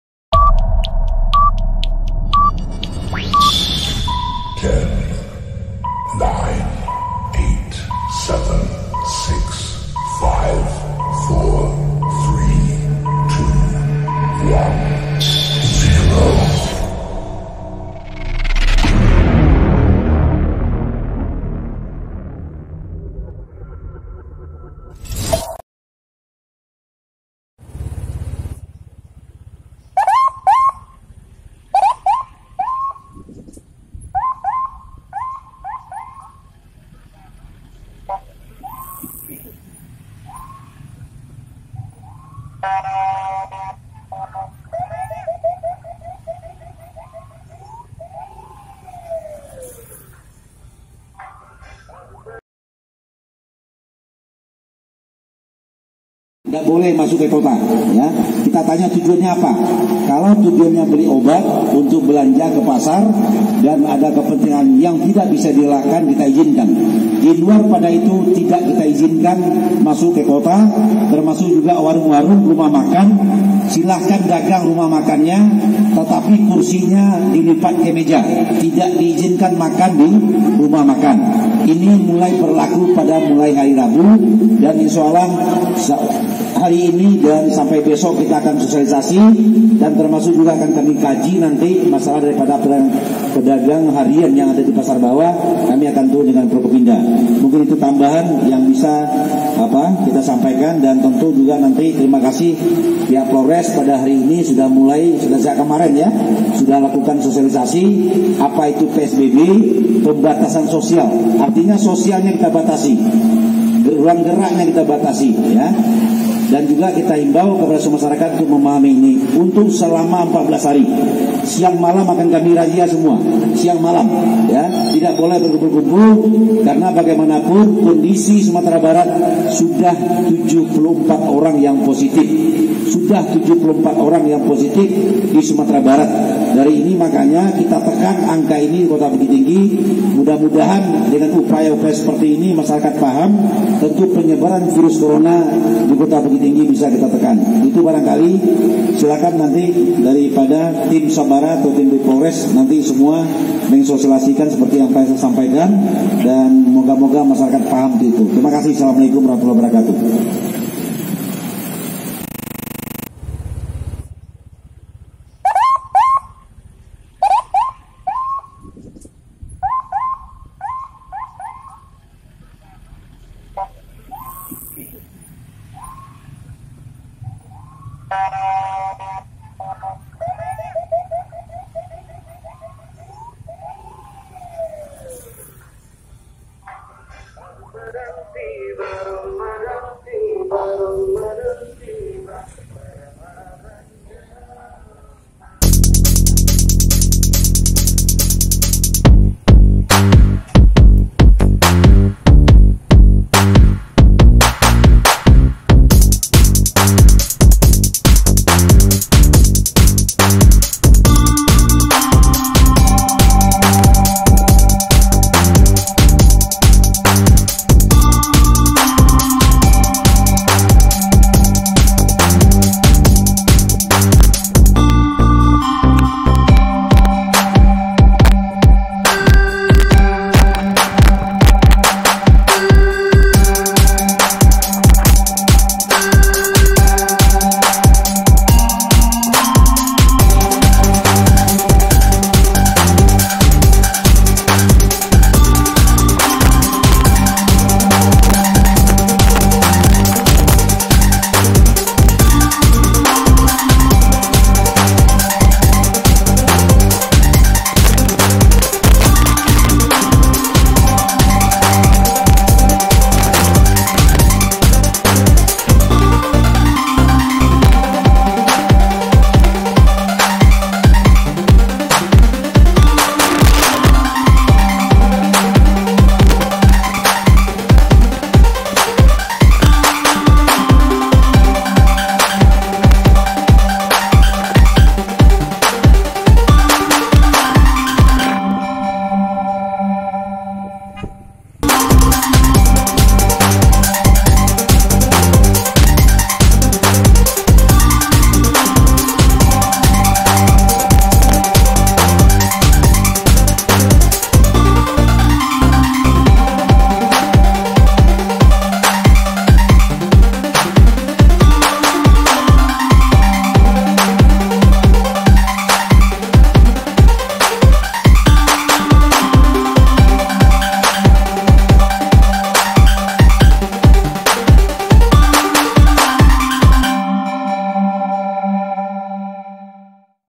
H-1 PEMBERLAKUAN PSBB DI KOTA BUKITTINGGI
PSBB adalah singkatan dari Pembatasan Sosial Berskala Besar, peraturan yang diterbitkan Kementerian Kesehatan (Kemenkes) dalam rangka Percepatan Penanganan COVID-19 agar bisa segera dilaksanakan di berbagai daerah. Aturan PSBB tercatat dalam Peraturan Menteri Kesehatan Nomor 9 Tahun 2020. Sekretaris Jenderal Kementerian Kesehatan Oscar Primadi dalam keterangan tertulisnya mengatakan PSBB melingkupi pembatasan sejumlah kegiatan penduduk tertentu dalam suatu wilayah yang diduga terinfeksi COVID-19 Khususnya Kota Bukittinggi